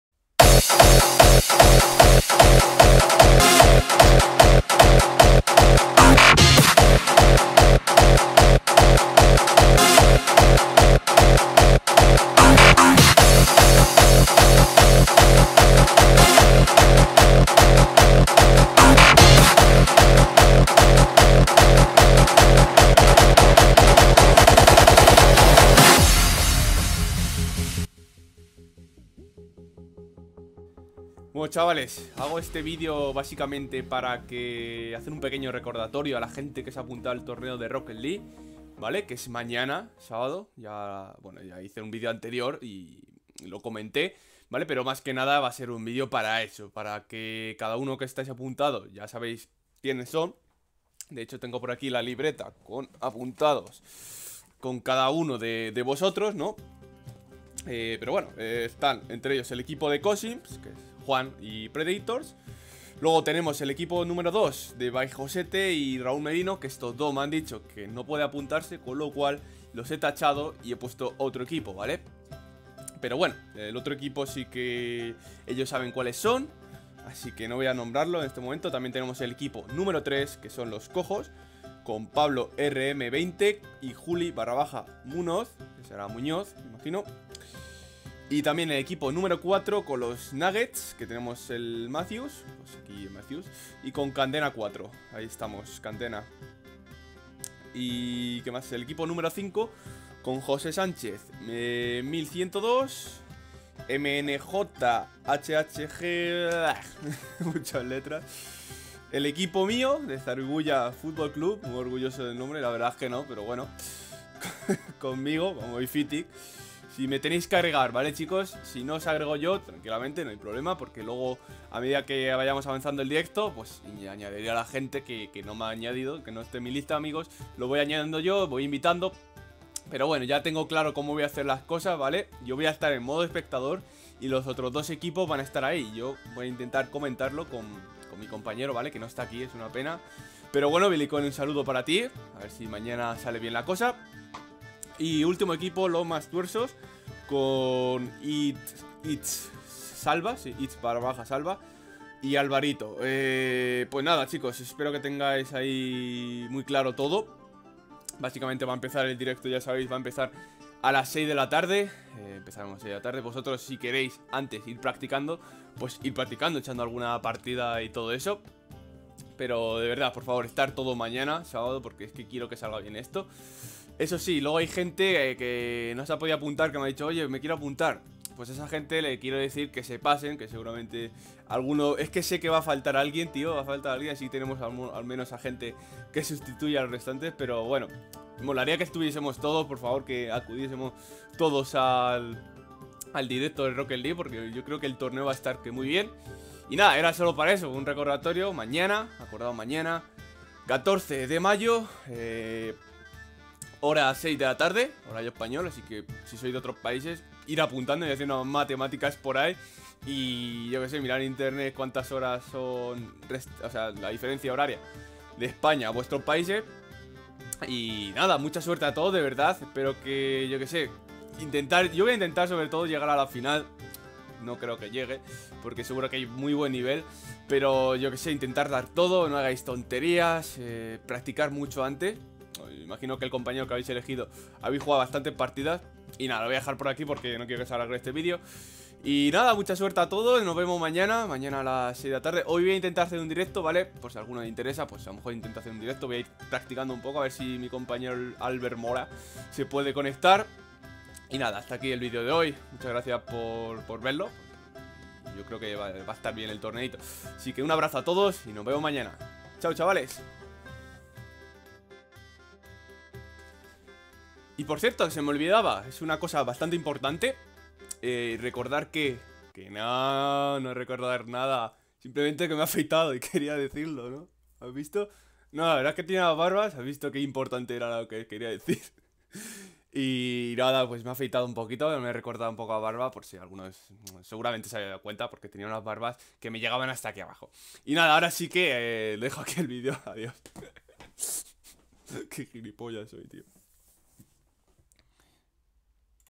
SE ACERCA EL TORNEO DE ROCKET LEAGUE (RECORDATORIO)
Vaaaaaaaaaamos ya queda nada mas y nada menos que un día! Espero que disfrutéis y que os lo paséis muy bien que es la intención. Ver el vídeo por favor, es importante!